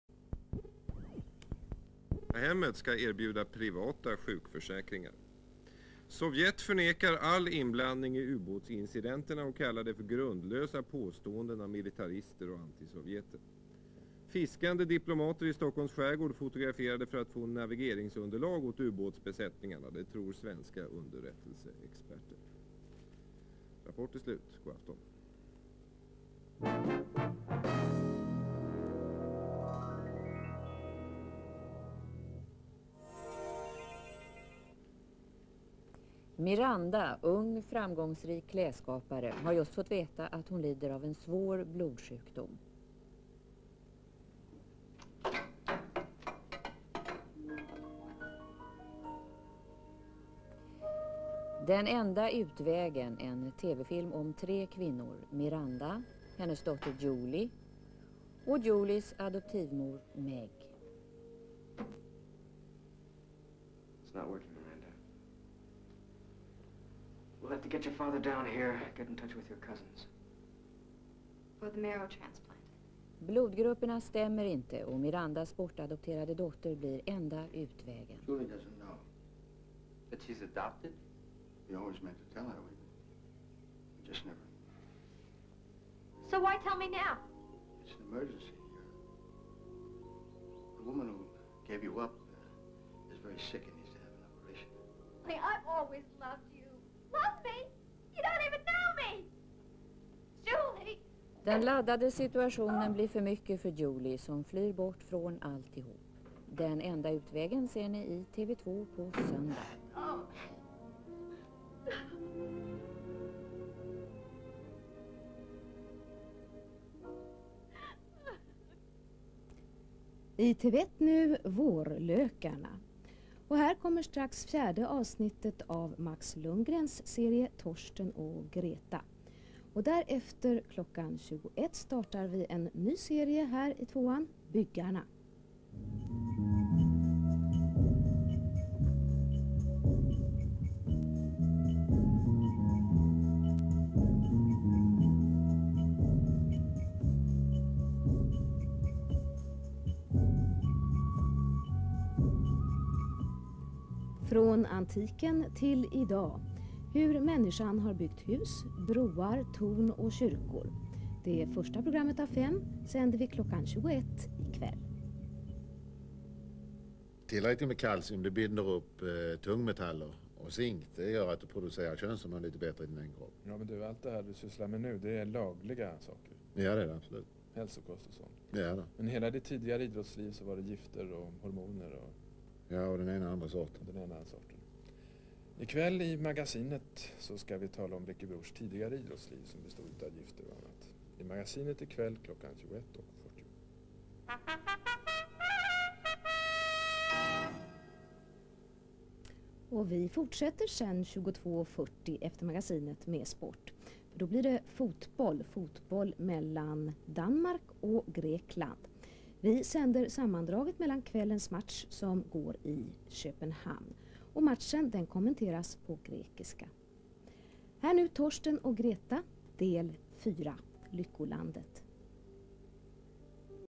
TV2-trailers + hallåa 1983-04-27
Några trailers samt TV2-hallåa Gunilla Linder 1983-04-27